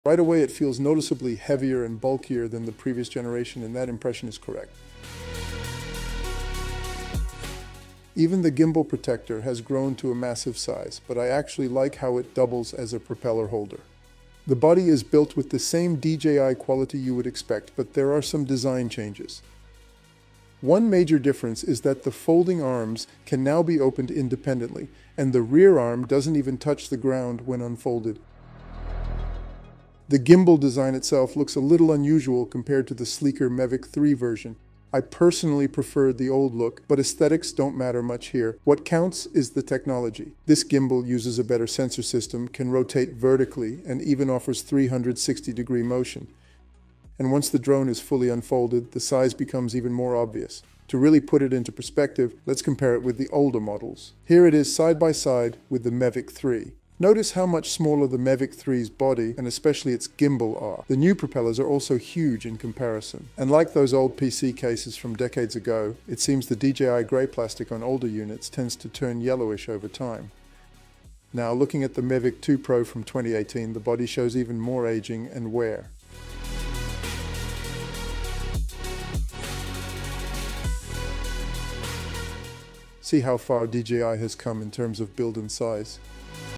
DJI Mavic 4 Pro: Unboxing and Size Comparison - Is It Worth It?
The latest DJI drone feels heavier and bulkier, marking a noticeable design shift. Folding arms now operate independently, and the gimbal boasts enhanced tech for superior performance. Comparing it to older models like the Mavic 3 reveals significant size differences.